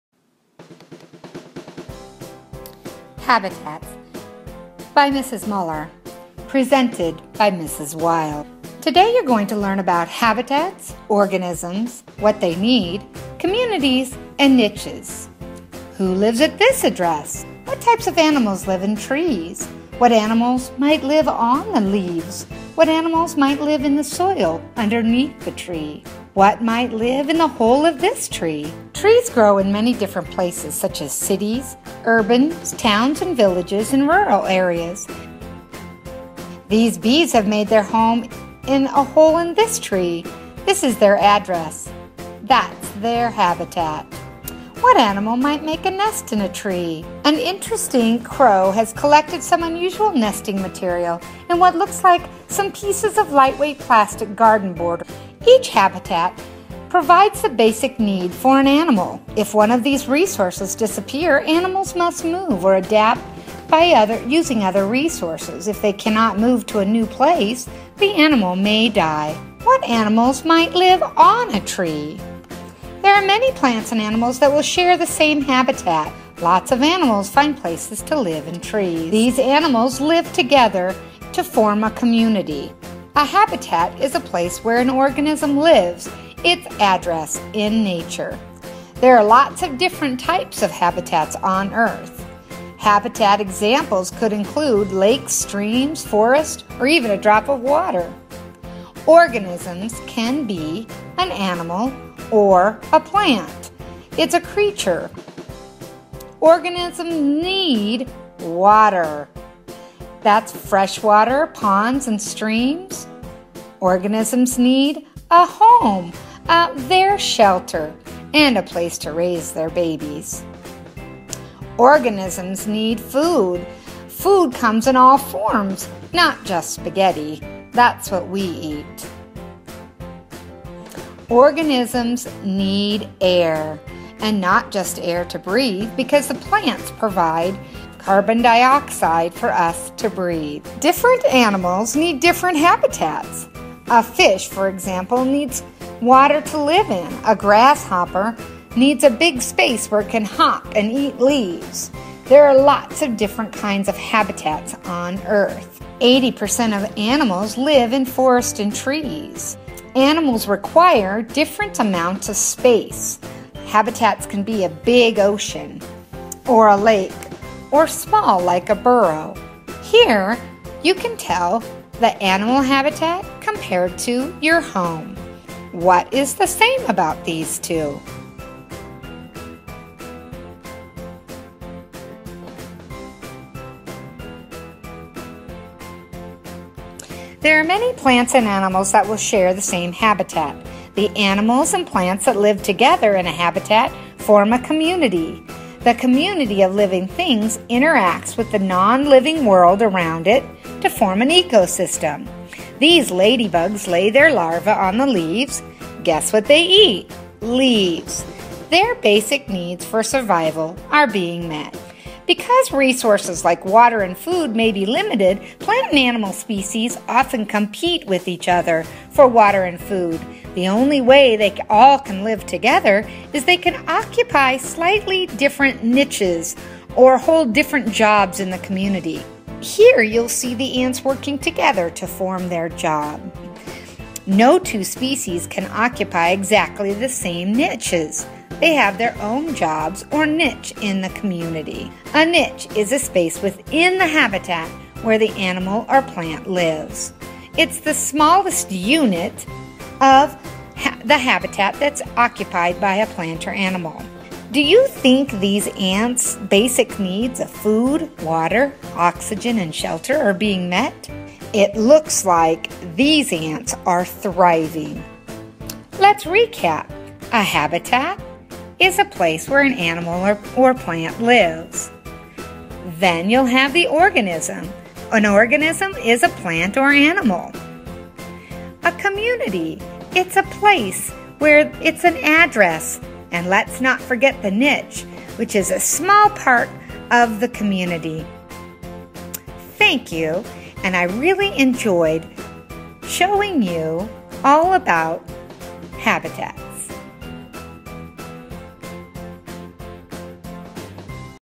Habitats
This video was created to teach 2nd graders that habitats provide animals what they need.  This video shows the special effects of animation within a video for Edtech 533.